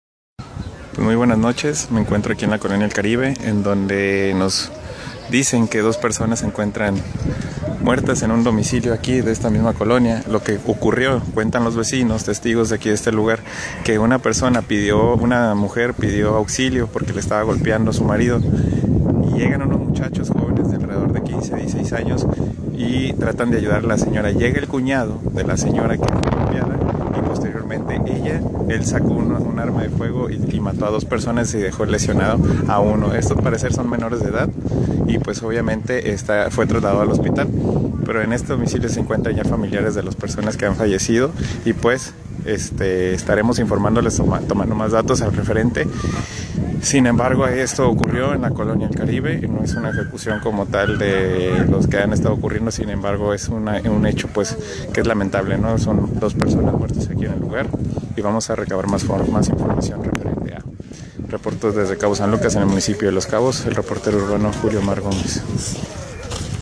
HOMICIDIO EN COL EL CARIBE!!!
Lamentable tragedia ocurrió a las 7 de la noche de ayer viernes 12 de junio de 2015, en colonia El Caribe de Cabo San Lucas, donde fueron asesinados a balazos dos menores de 16 años. (Video Julio Omar Gómez/El Reportero Urbano).